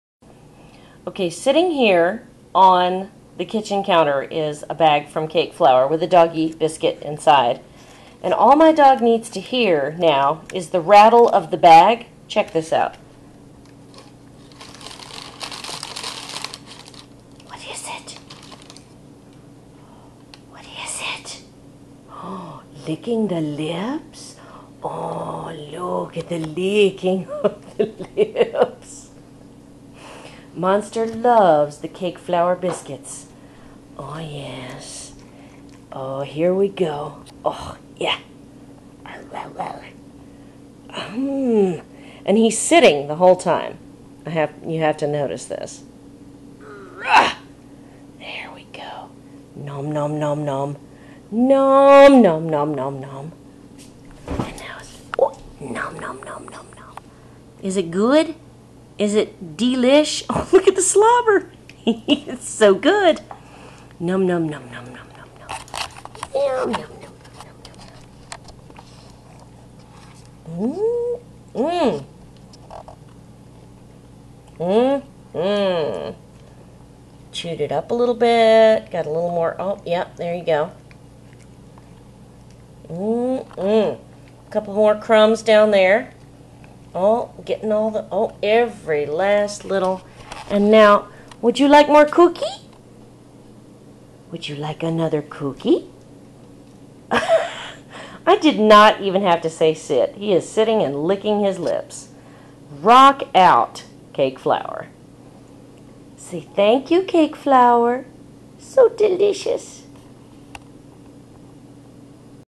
Monster eating a Cake Flour biscuit! Oh, nom nom nom.
Monster Frahnkensteen Jackson eating a biscuit from Cake Flour.  He does not wait for anyone to say sit.  Nom, nom, nom.